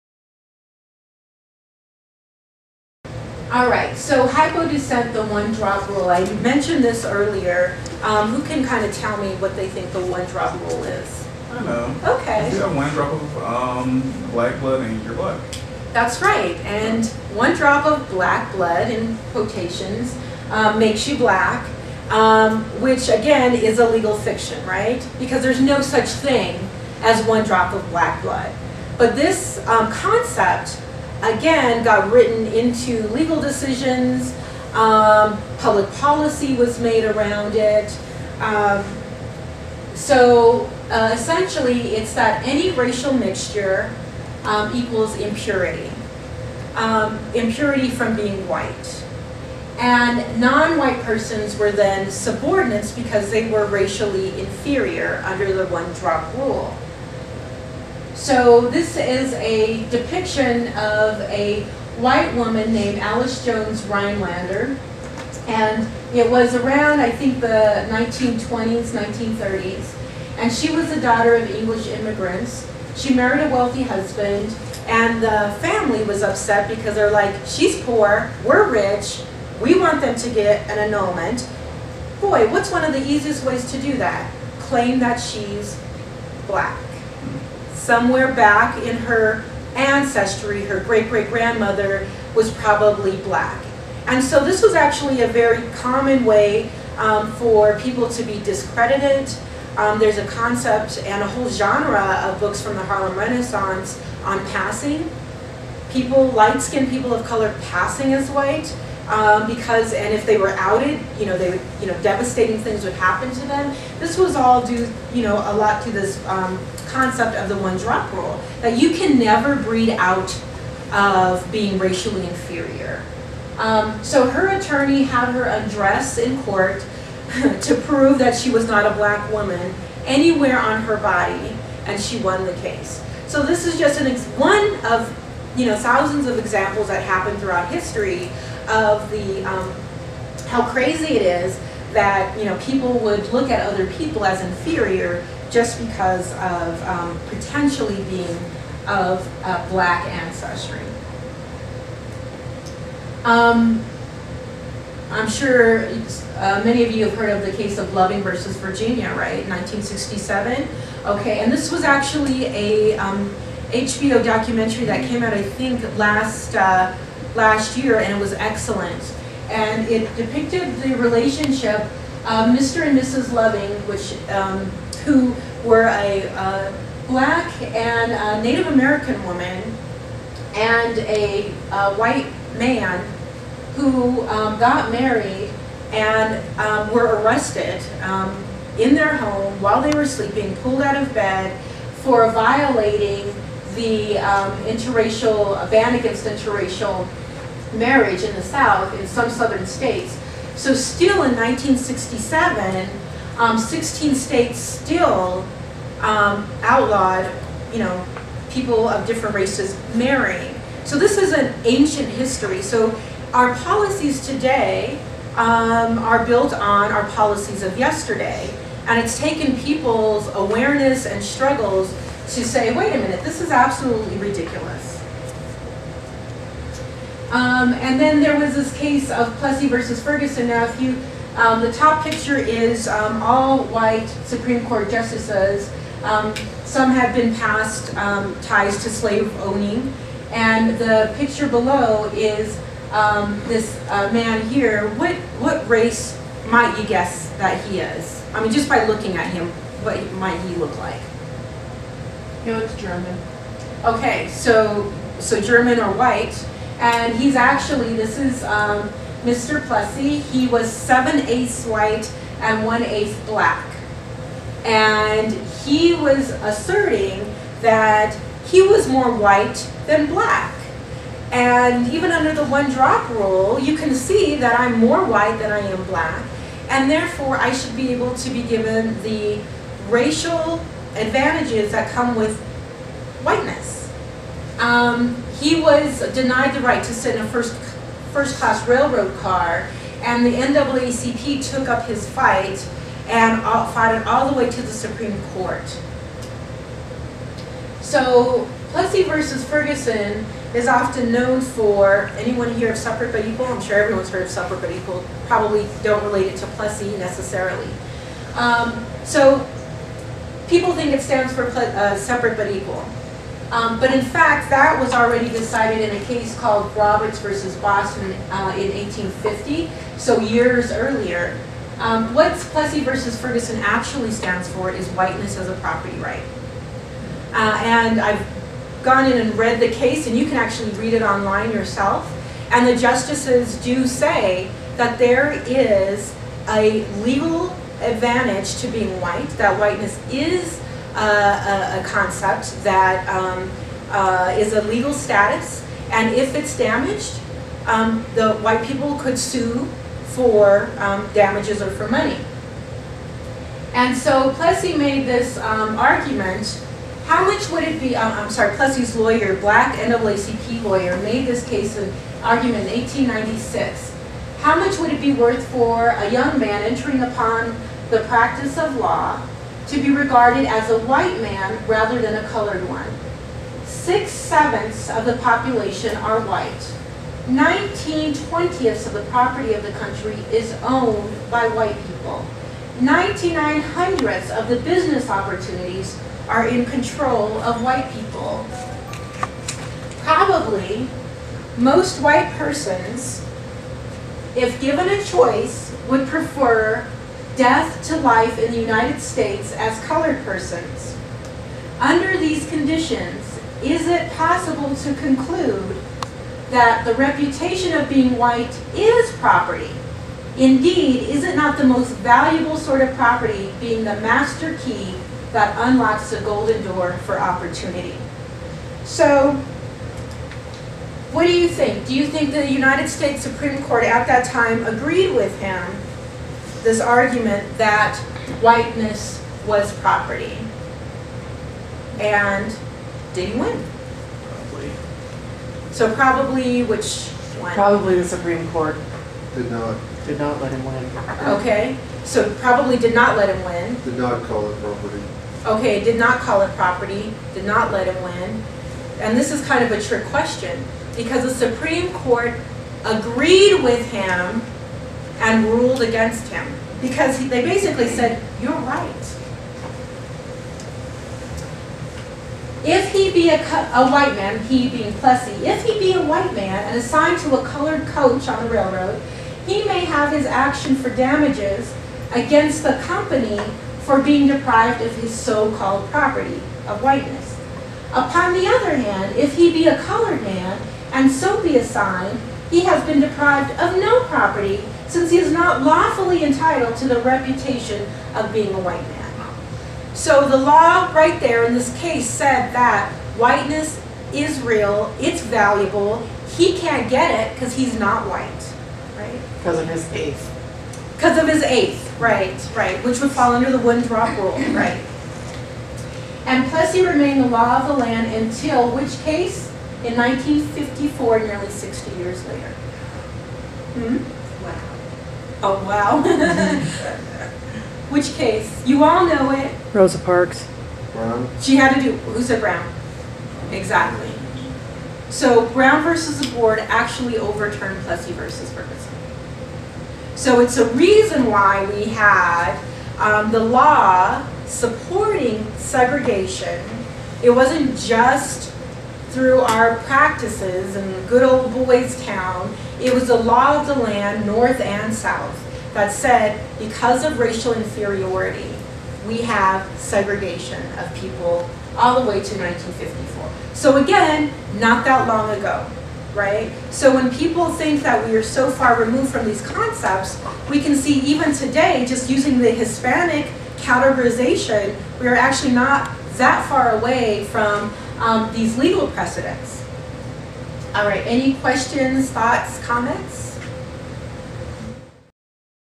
Lisa Calderon One drop rule
In the 1800's the US and others often adopted a "one drop rule" in regards of defining the race of persons.